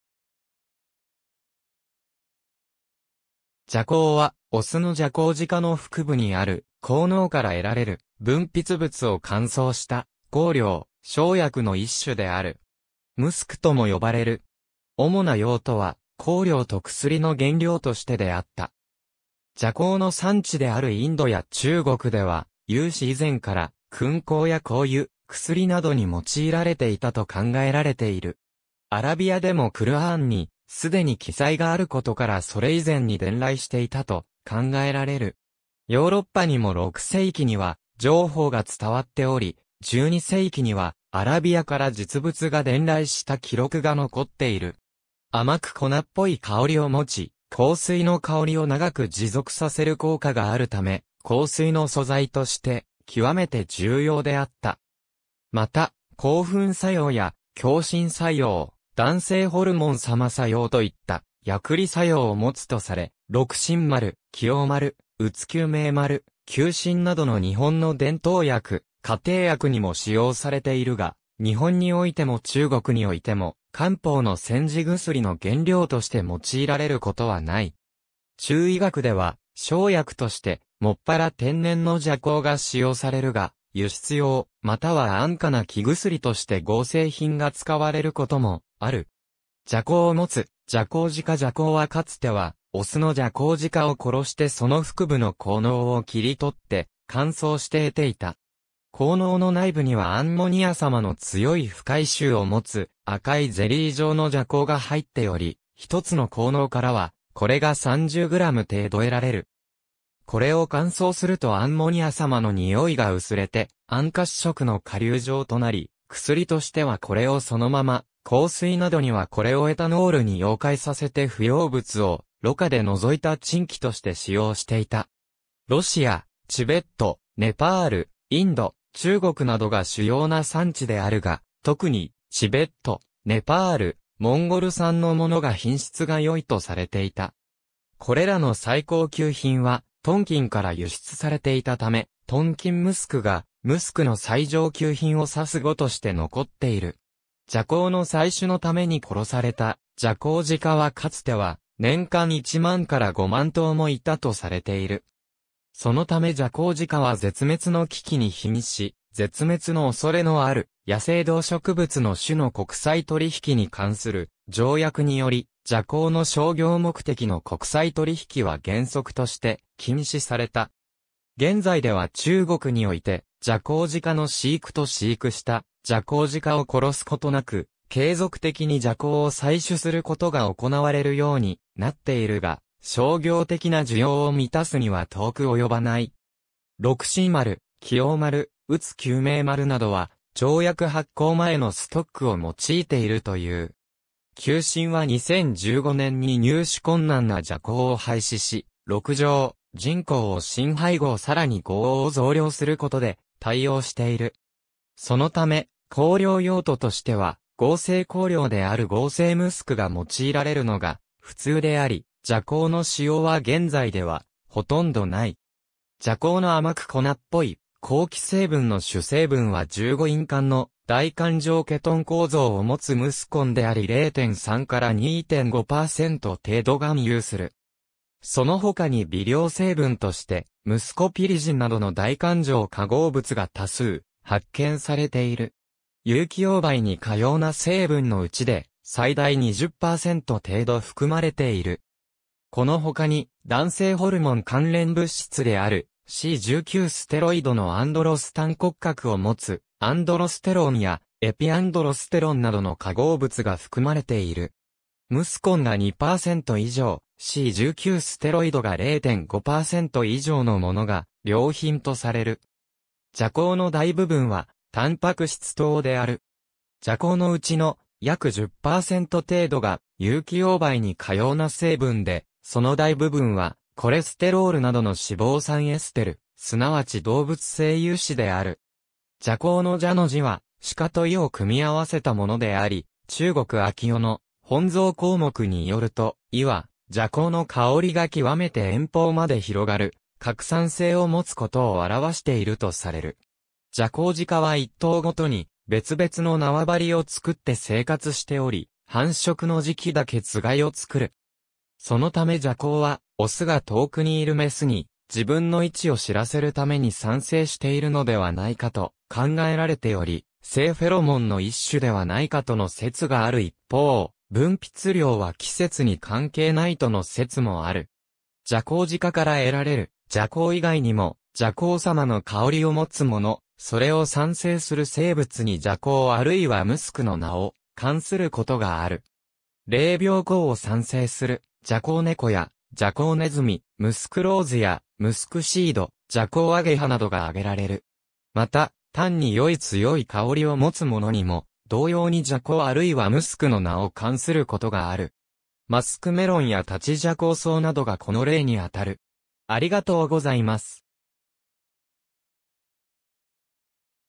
麝香
麝香（じゃこう）は雄のジャコウジカの腹部にある香嚢（ジャコウ腺）から得られる分泌物を乾燥した香料、生薬の一種である。
ムスク（英: musk）とも呼ばれる。
主な用途は香料と薬の原料としてであった。
麝香の産地であるインドや中国では有史以前から薫香や香油、薬などに用いられていたと考えられている。
アラビアでもクルアーンにすでに記載があることからそれ以前に伝来していたと考えられる。
ヨーロッパにも6世紀には情報が伝わっており、12世紀にはアラビアから実物が伝来した記録が残っている。
甘く粉っぽい香りを持ち、香水の香りを長く持続させる効果があるため、香水の素材として極めて重要であった。
また、興奮作用や強心作用、男性ホルモン様作用といった薬理作用を持つとされ、六神丸、奇応丸、宇津救命丸、救心などの日本の伝統薬・家庭薬にも使用されているが、日本においても中国においても漢方の煎じ薬の原料として用いられることはない。
中医学では生薬として、専ら天然の麝香が使用されるが、輸出用、または安価な生薬として合成品が使われることもある。
麝香をもつジャコウジカ （シベリアジャコウジカ） 麝香はかつては雄のジャコウジカを殺してその腹部の香嚢を切り取って乾燥して得ていた。
香嚢の内部にはアンモニア様の強い不快臭を持つ赤いゼリー状の麝香が入っており、一つの香嚢からはこれが30グラム程度得られる。
これを乾燥するとアンモニア様の臭いが薄れて暗褐色の顆粒状となり、薬としてはこれをそのまま、香水などにはこれをエタノールに溶解させて不溶物を濾過で除いたチンキとして使用していた。
ロシア、チベット、ネパール、インド、中国などが主要な産地であるが、特にチベット、ネパール、モンゴル産のものが品質が良いとされていた。
これらの最高級品はトンキンから輸出されていたため、トンキン・ムスクがムスクの最上級品を指す語として残っている。
麝香の採取のために殺されたジャコウジカはかつては年間1万から5万頭もいたとされている。
そのためジャコウジカは絶滅の危機に瀕し、絶滅のおそれのある野生動植物の種の国際取引に関する条約（ワシントン条約）によりジャコウの商業目的の国際取引は原則として禁止された。
現在では中国においてジャコウジカの飼育と飼育したジャコウジカを殺すことなく継続的に麝香を採取すること（麻酔で眠らせるなどの方法がある）が行なわれるようになっているが、商業的な需要を満たすには遠く及ばない。
六神丸、奇応丸、宇津救命丸などは条約発効前のストックを用いているという。
救心は2015年に入手困難な麝香を廃止し、鹿茸、沈香を新配合さらに牛黄を増量することで対応している。
そのため、香料用途としては合成香料である合成ムスクが用いられるのが普通であり、麝香の使用は現在ではほとんどない。
麝香の甘く粉っぽい香気成分の主成分は15員環の大環状ケトン構造をもつムスコン（3 - メチルシクロペンタデカノン）であり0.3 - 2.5% 程度含有する。
そのほかに微量成分としてムスコピリジン (muscopyridine) などの大環状化合物が多数発見されている。
有機溶媒に可溶な成分のうちで最大20%程度含まれている。
この他に男性ホルモン関連物質であるC19-ステロイドのアンドロスタン骨格を持つアンドロステロンやエピアンドロステロン (epiandrosterone) などの化合物が含まれている。
ムスコンが2% 以上、C19-ステロイドが0.5% 以上のものが良品とされる。
麝香の大部分はタンパク質等である。
麝香のうちの約10%程度が有機溶媒に可溶な成分で、その大部分はコレステロールなどの脂肪酸エステル、すなわち動物性油脂である。
麝香の麝の字は鹿と射を組み合わせたものであり、中国明代の『本草綱目』によると、射は麝香の香りが極めて遠方まで広がる拡散性を持つことを表しているとされる。
ジャコウジカは一頭ごとに別々の縄張りを作って生活しており、繁殖の時期だけつがいを作る。
そのため麝香は雄が遠くにいる雌に自分の位置を知らせるために産生しているのではないかと考えられており、性フェロモンの一種ではないかとの説がある一方、分泌量は季節に関係ないとの説もある。
ジャコウジカから得られる麝香以外にも、麝香様の香りを持つもの、それを産生する生物に麝香あるいはムスクの名を冠することがある。
霊猫香（シベット）を産生するジャコウネコやジャコウネズミ、ムスクローズやムスクシード（アンブレットシード）、ジャコウアゲハなどが挙げ...